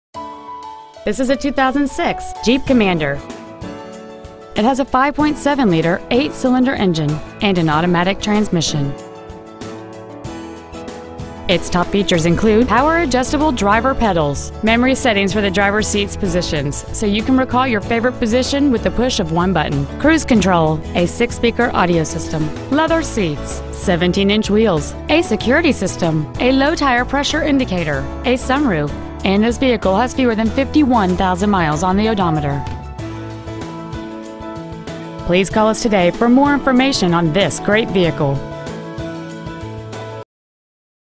2006 JEEP COMMANDER AZ
We've been honored to serve the  AZ area , we promise that your experience at our dealership will exceed your expectations !

 Year : 2006

 Make : JEEP

 Model : COMMANDER

 Engine : 5.7L V8

 Trans . : AUTO

 Exterior : SILVER

 Miles : 50,676

 Interior : BEIGE

 Earnhardt Ford

 7300 West Orchid Lane

 2006 JEEP COMMANDER  AZ